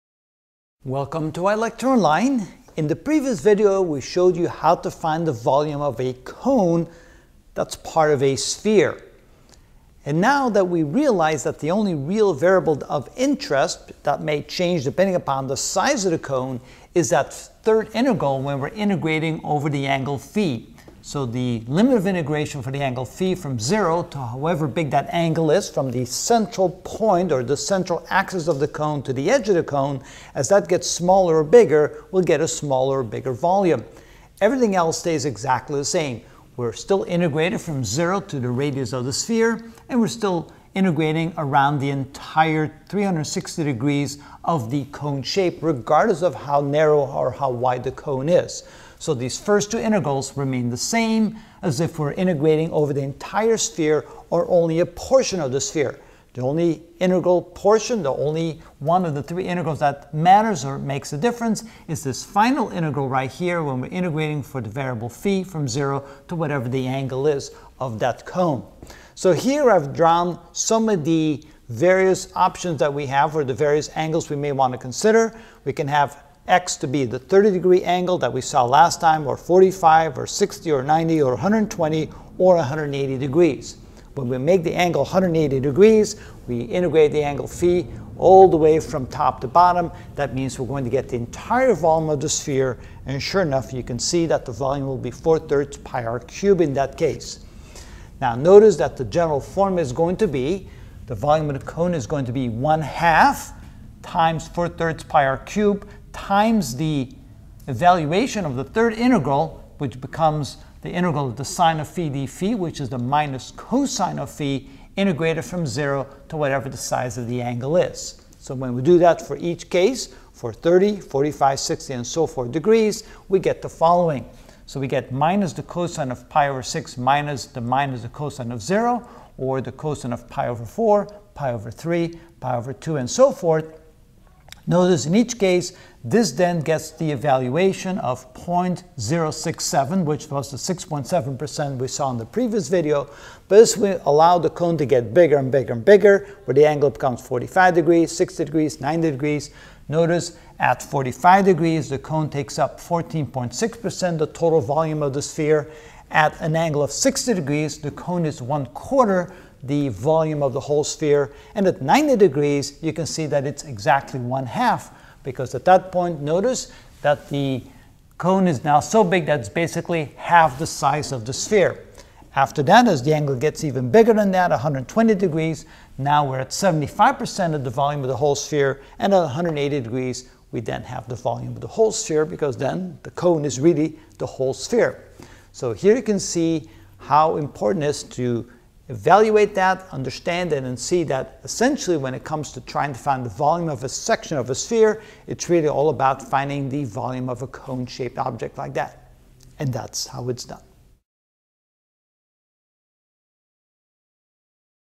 Calculus 3: Triple Integrals (7 of 25) Finding the Volume of a Cone: Part 2: Spherical
In this video I will show how the volume of a cone changes when the angle phi changes from 30 degrees to 180 degrees.

Next video in this series can be seen at: